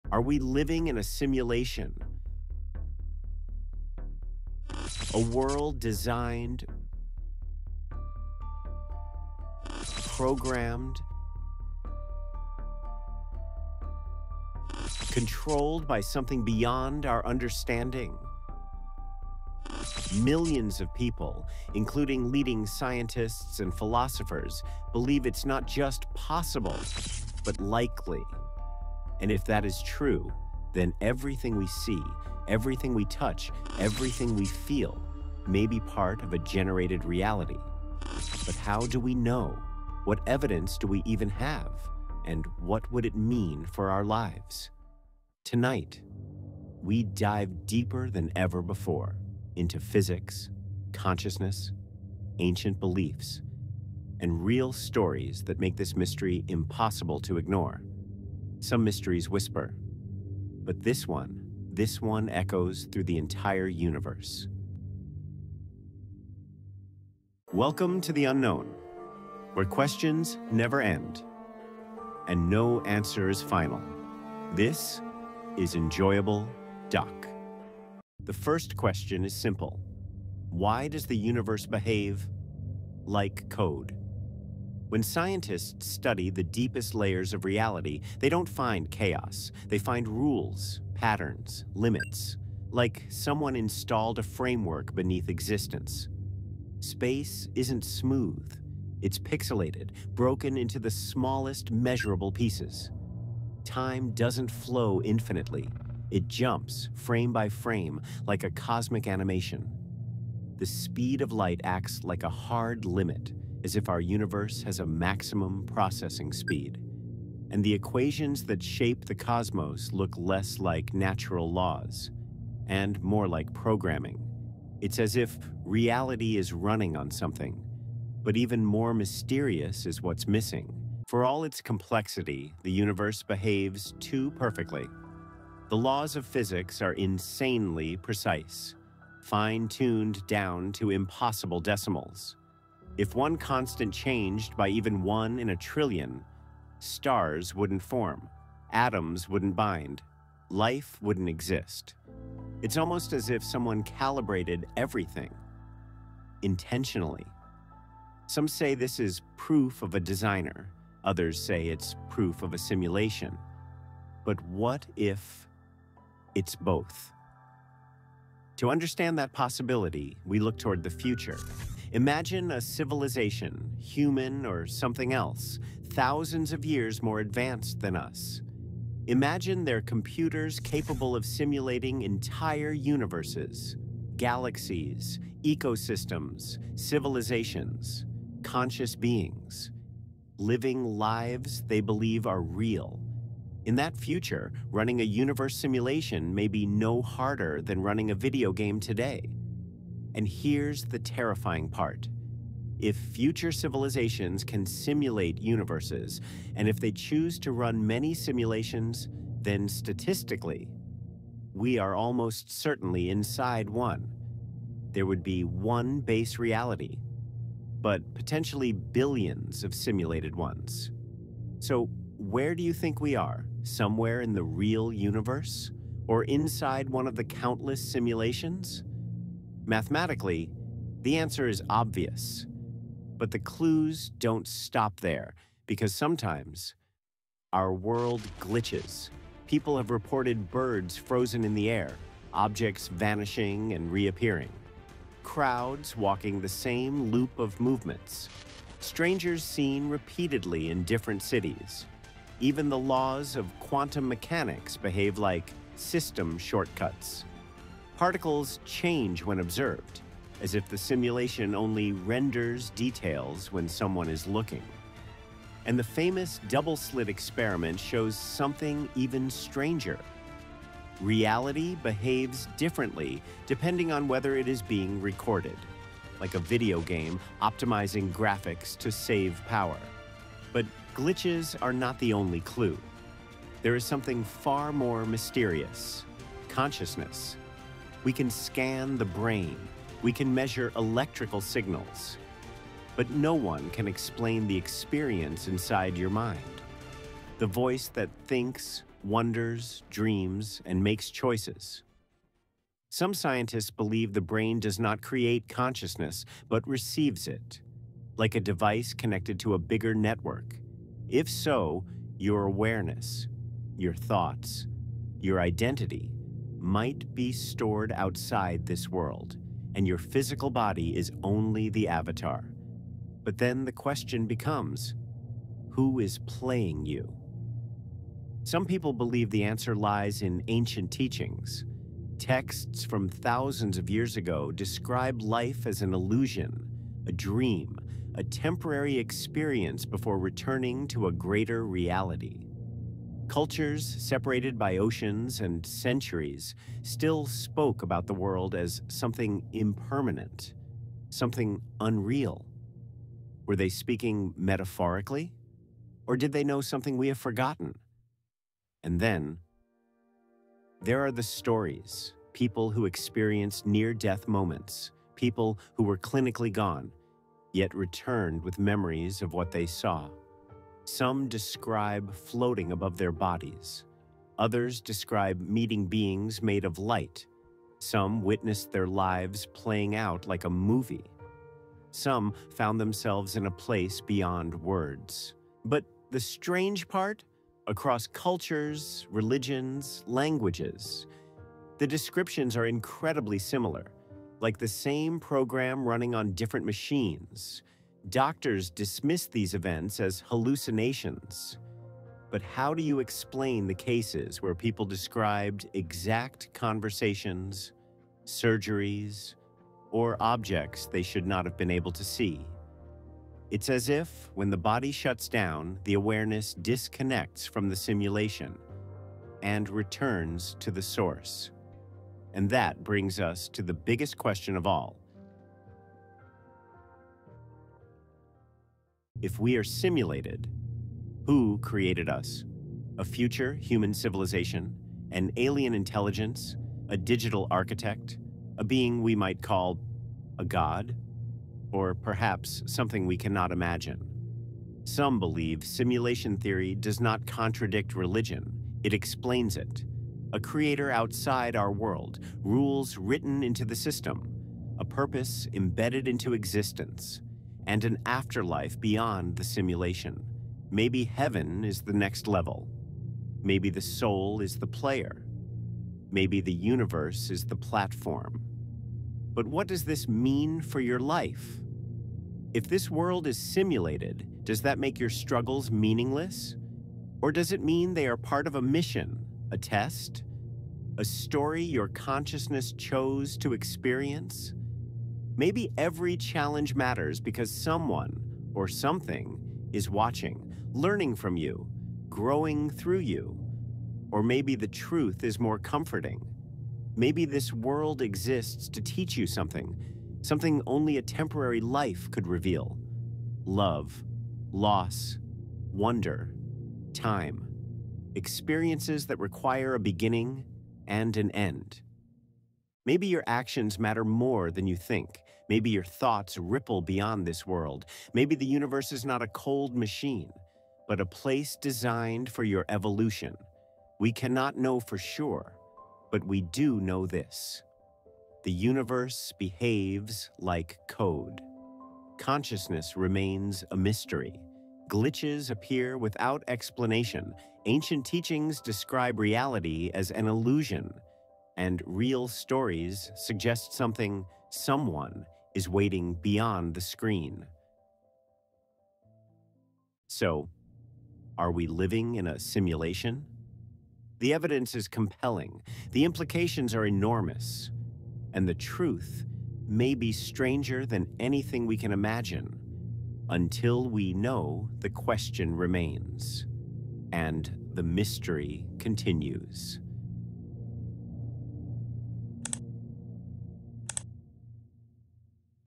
What If Nothing in Your Life Is Real?
Are we living in a simulation? For decades, scientists, philosophers, and tech visionaries have questioned whether our reality is real—or a highly advanced program created by an intelligent civilization. In this video, we explore the most convincing evidence behind the Simulation Theory: physics glitches, mathematical patterns in nature, quantum behavior, digital structures in space-time, and the possibility that consciousness itself may be a receiver of information rather than a creator of it.

From the Mandela Effect to déjà vu, from quantum particles behaving like code to NASA’s discoveries about the universe functioning like a 3D hologram—this deep-dive will show you why so many experts believe we might not be living in a physical world, but inside an advanced simulation far beyond our understanding.

If you’re interested in the nature of reality, the origin of consciousness, artificial intelligence, futuristic civilizations, parallel universes, or the mysteries of existence, this video is for you. Prepare to see the world differently.

✨ LIKE if you enjoy mind-bending science